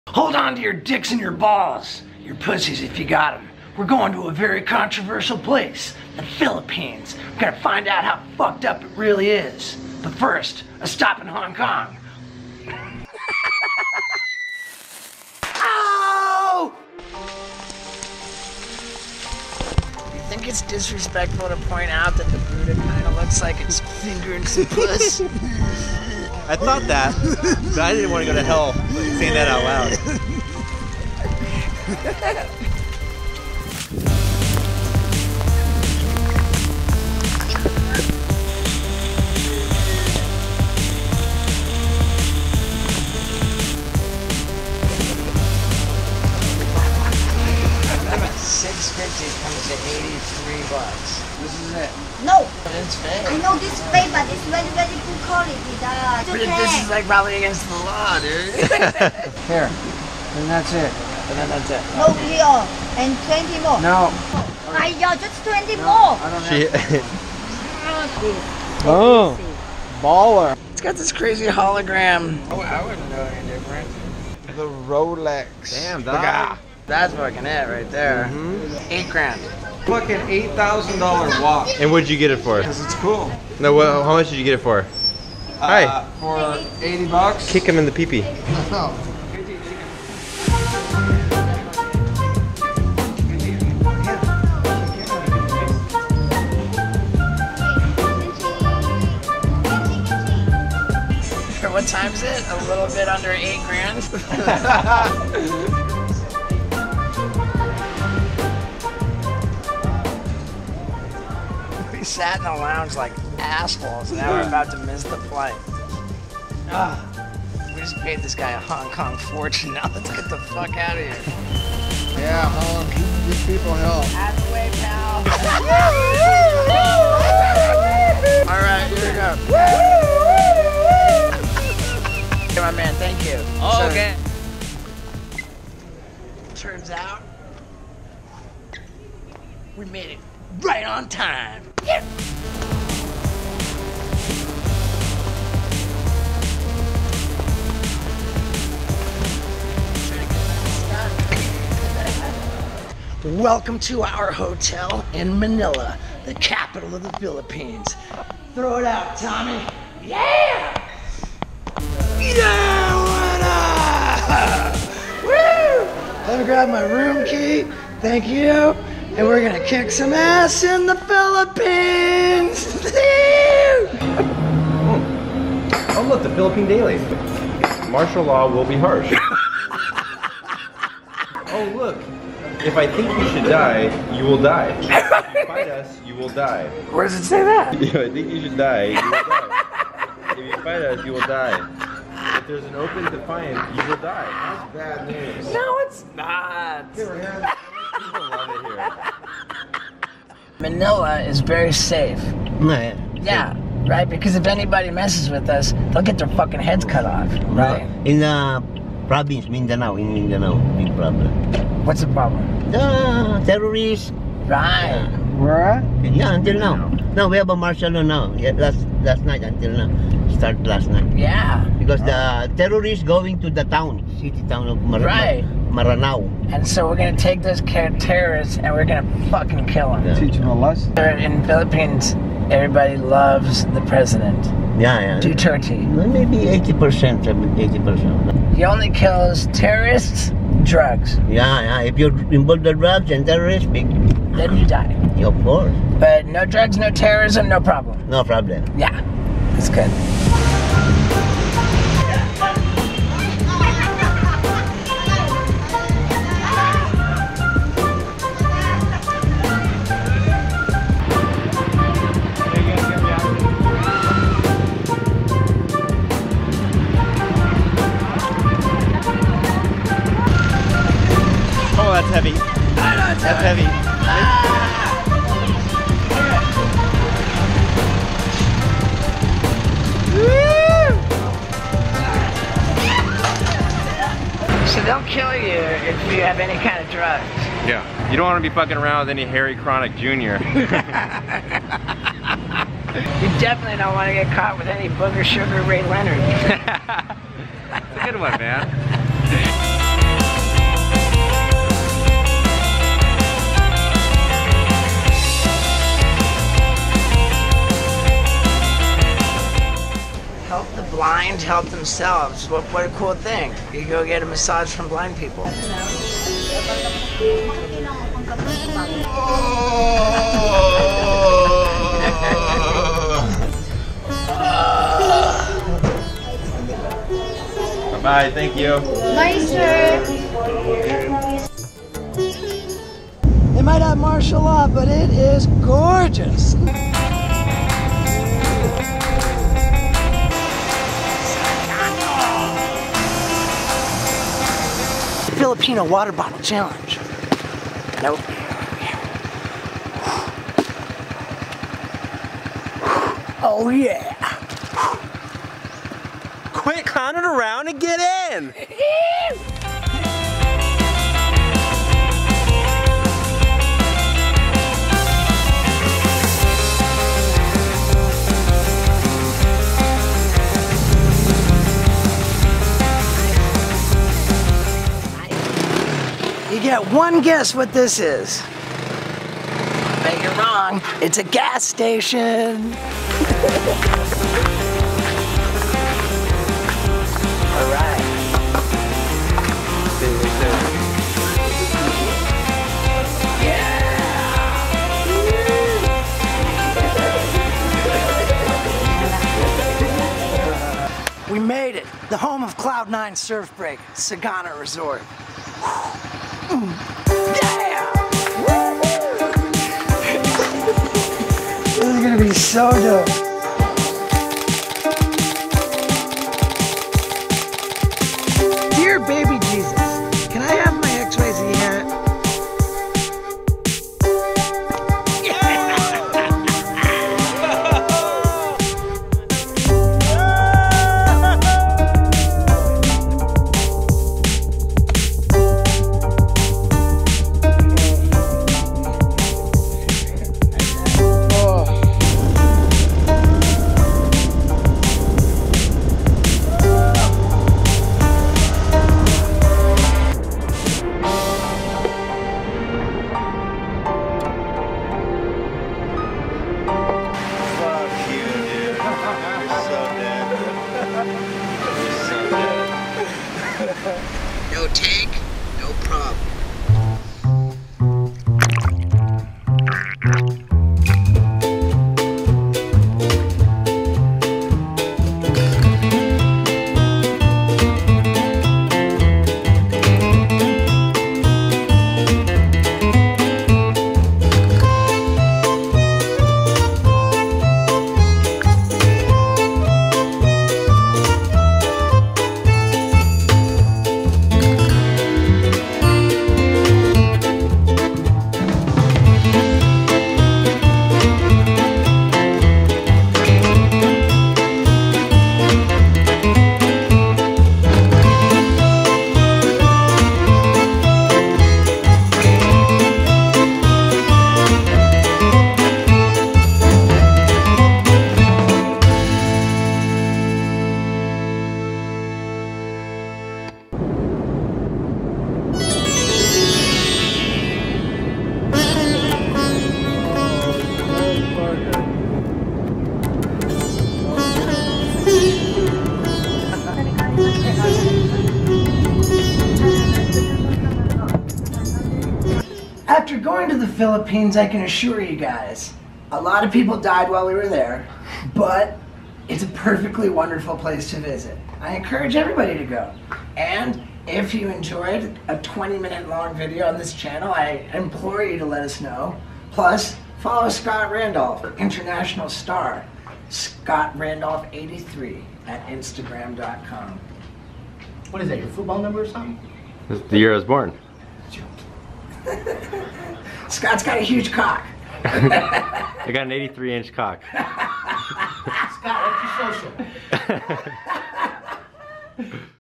If I Think You Should Die, You Will Die | Steve-O TV
We went to Asia and things got really crazy. Give this video lots of love if you want me to make more... Thanks!